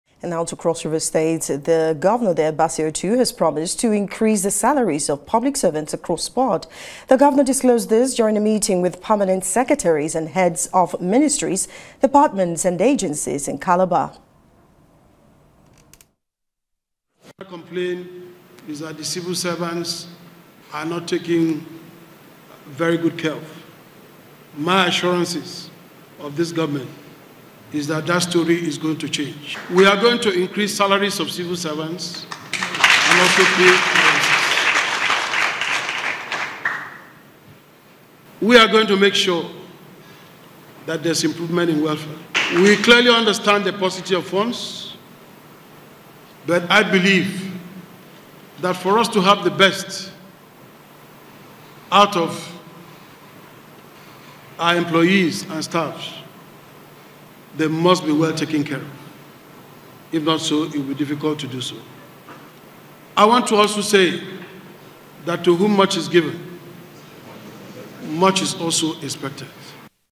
Governor Otu to increase salaries of civil and public servants
Video from Magdalene